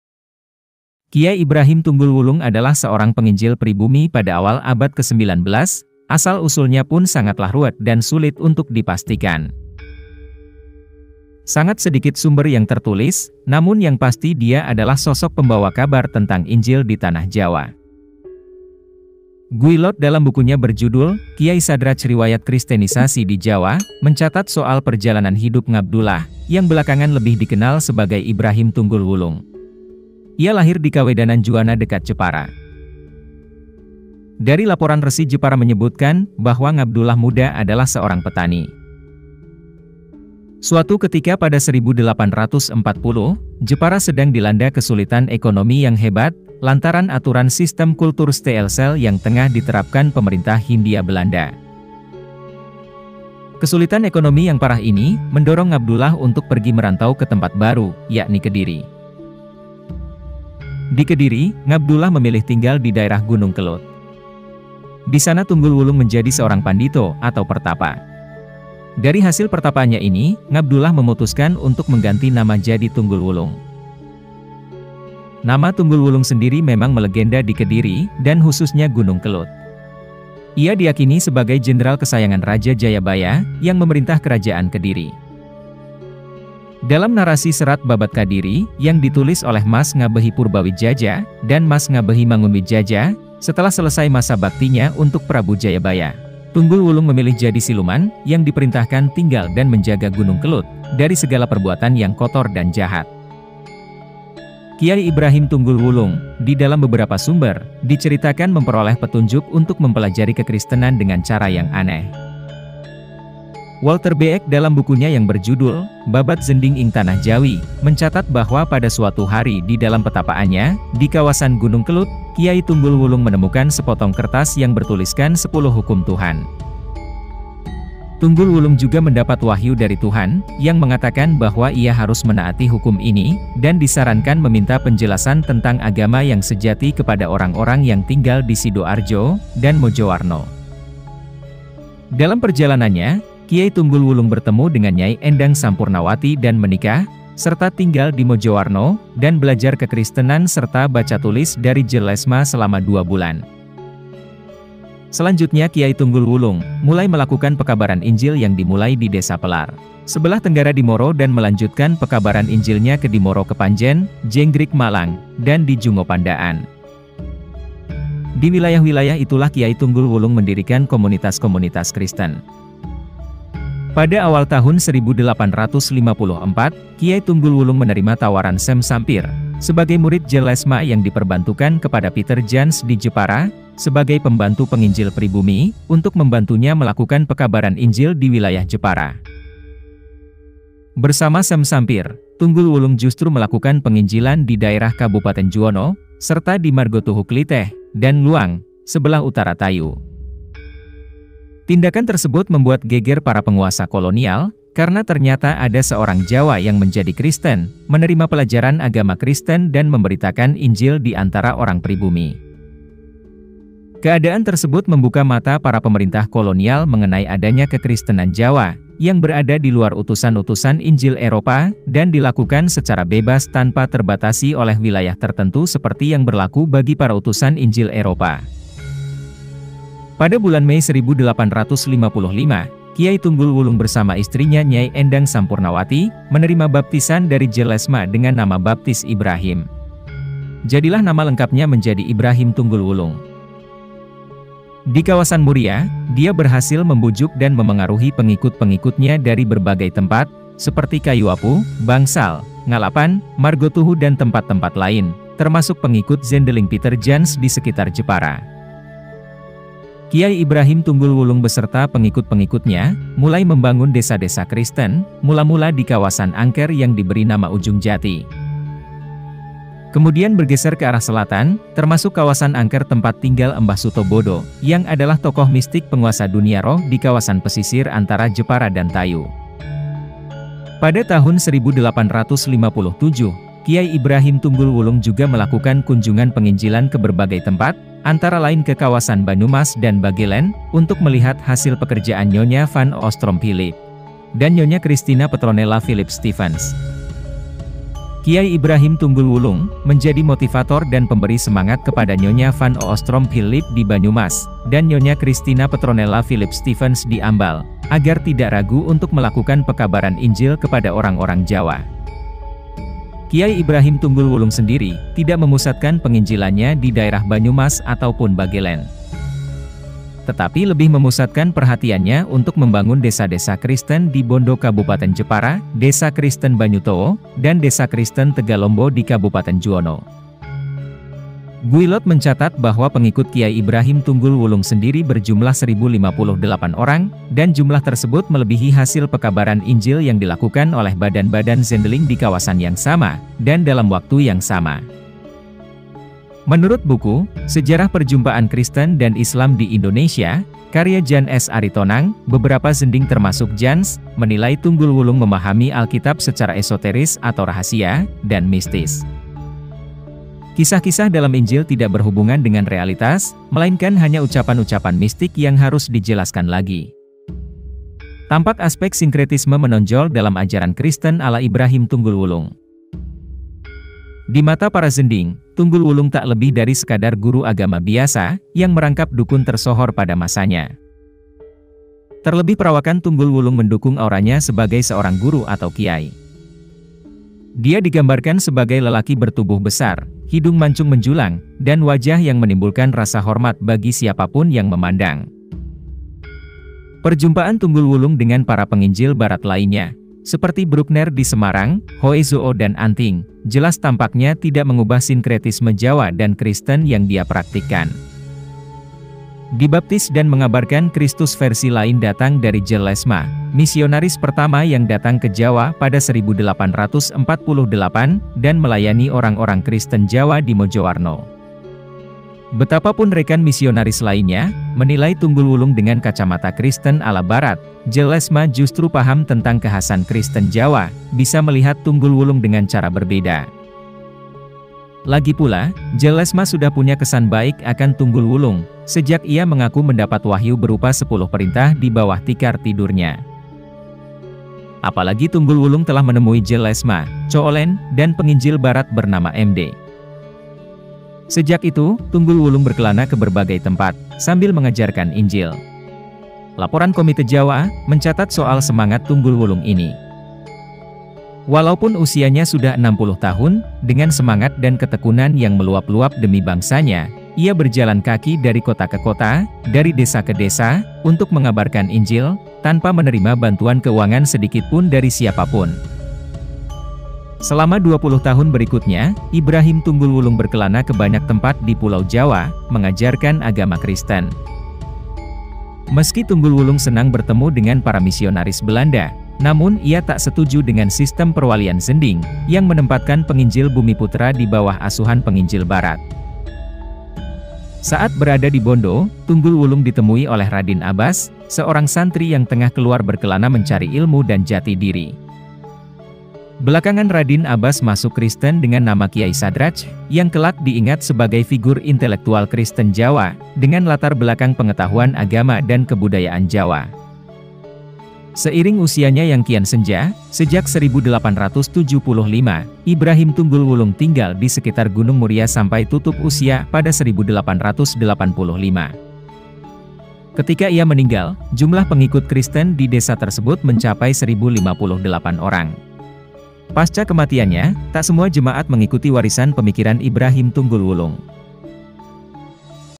Kisah Kiai Tunggul Wulung Penginjil Pribumi Dari Lereng Gunung Kelud
Kiai Tunggul Wulung bahkan pernah menggegerkan penguasa kolonial dengan aksinya yang memberi pengabaran Injil kepada rakyat pribumi tanpa sepengetahuan pemerintah kala itu.

C. Guillot dalam bukunya berjudul ‘Kiai Sadrach Riwayat Kristenisasi di Jawa’ mencatat soal perjalanan hidup Ngabdullah yang belakangan lebih dikenal sebagai Ibrahim Tunggul Wulung. Ia lahir di Kawedanan Juwana dekat Jepara.